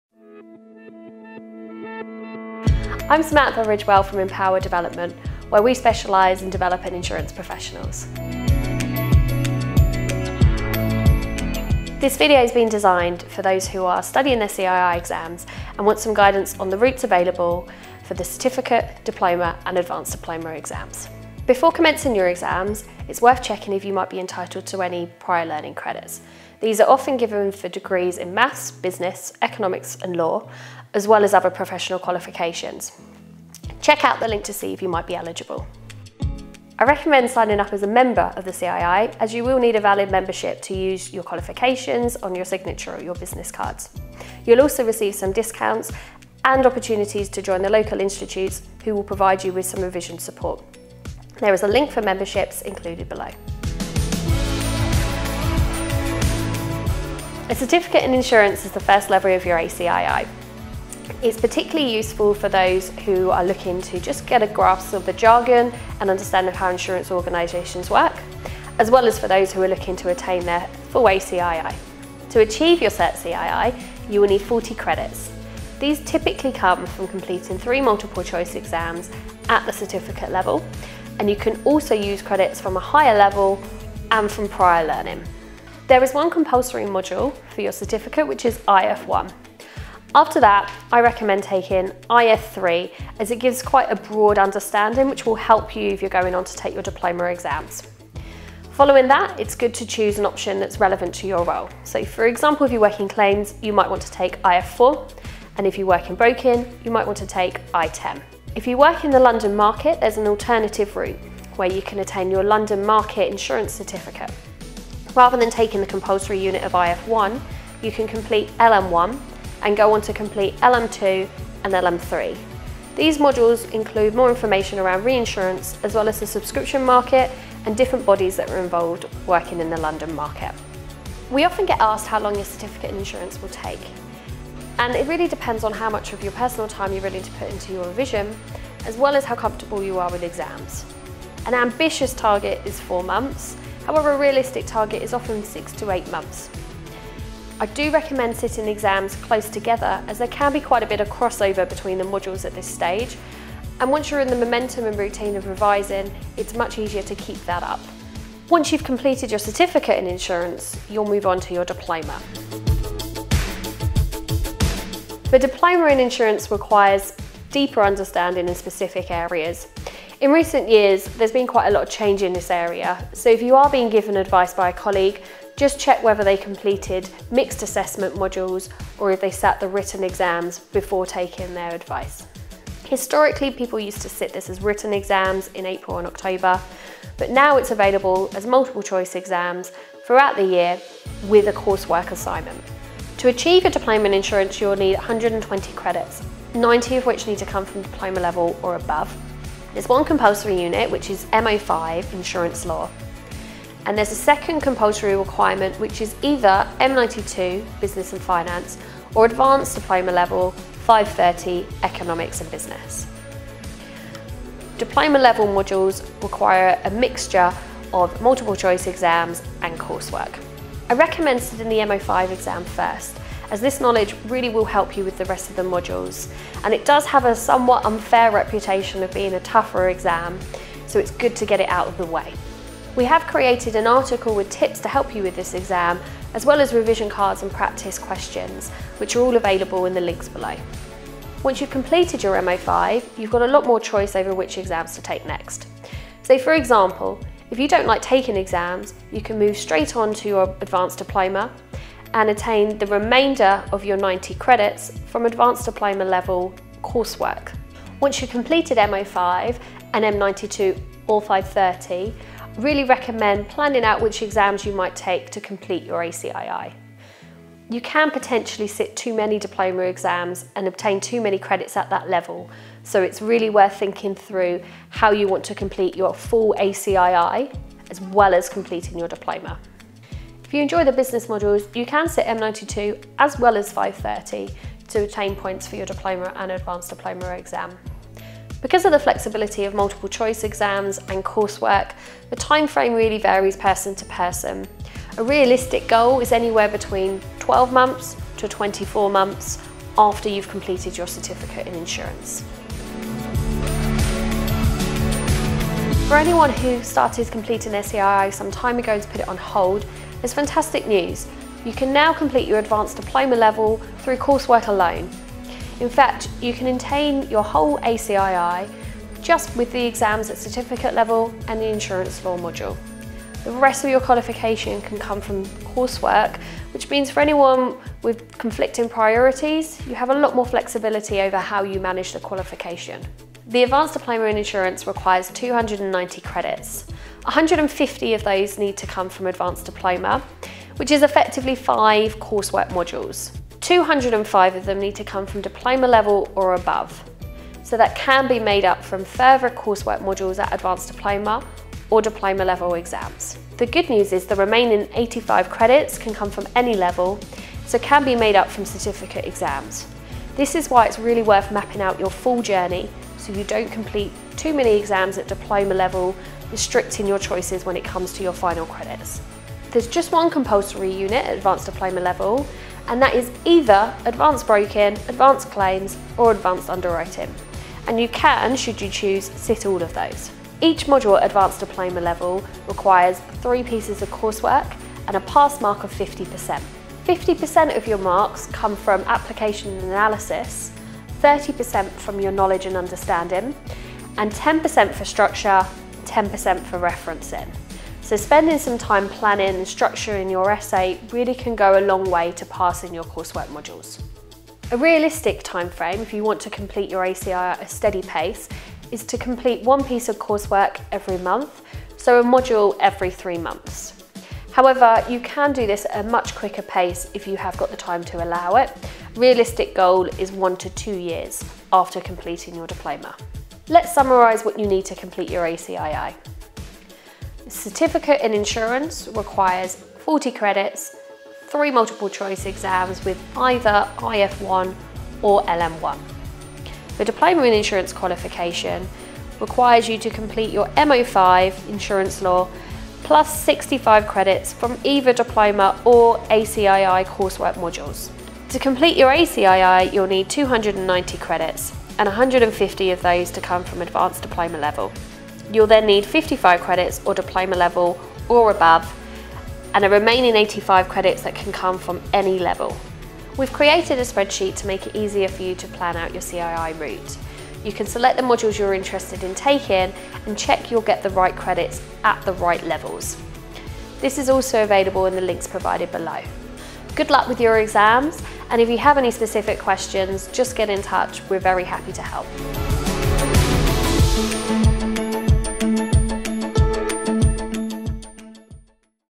ACII Completing Your Advanced Diploma
Sam talks you through the routes available for the Certificate, Diploma & Advanced Diploma exams.

Here is the link to the CII Workbook for calculating required credits and compulsory modules

0:00 Introduction
0:10 Your CII Journey
0:21 Prior Learning Credits
0:55 Certificate In Insurance (Cert CII)
1:12 Cert CII - Credits & Exams
1:48 The London Market Route
2:11 Timeframe to Complete Cert CII
2:44 Diploma In Insurance (Dip CII)
3:13 Dip CII - Credits, Exams & Coursework
4:48 Timeframe to Complete Dip CII
5:04 Advanced Diploma (ACII)
5:33 ACII - Credits, Exams & Coursework
6:44 Timeframe to Complete ACII
7:03 Summary for Completing ACII